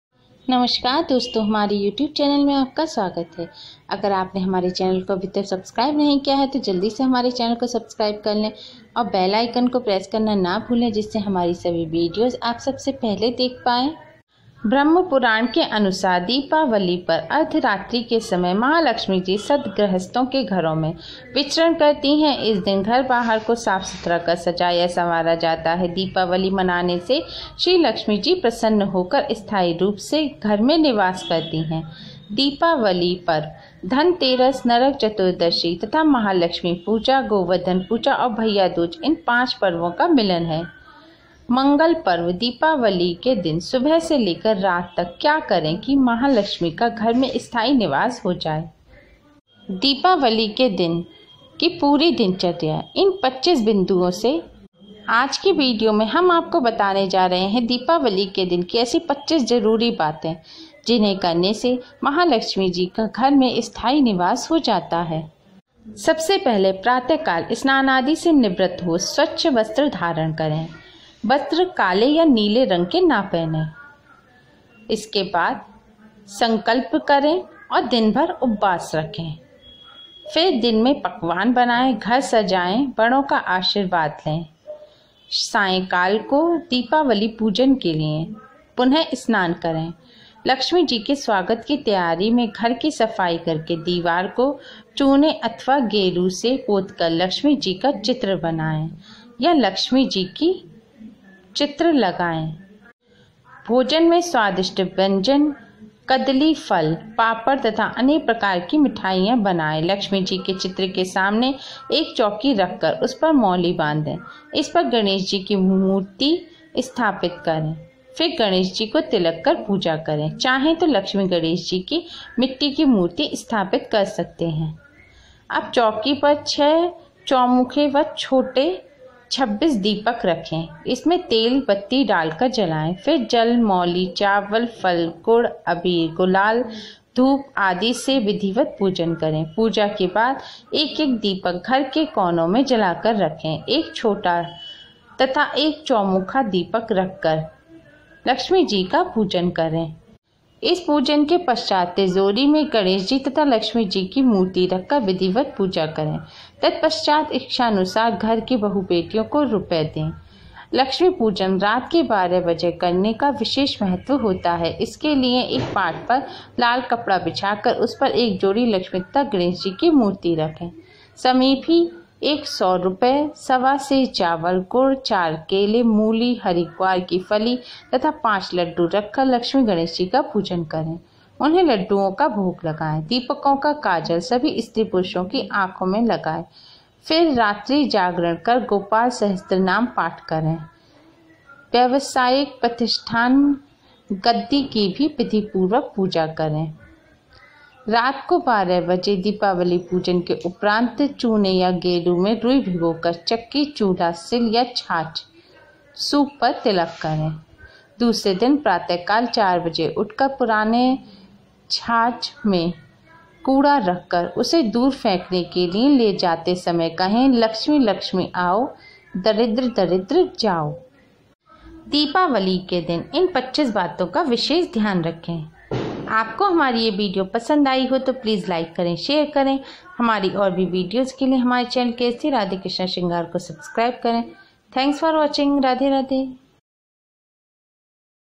दिवाली पर क्या करें, सुबह से लेकर शाम तक की 25 जरूरी बातें
राधे राधे लिखकर कमेंट जरुर करे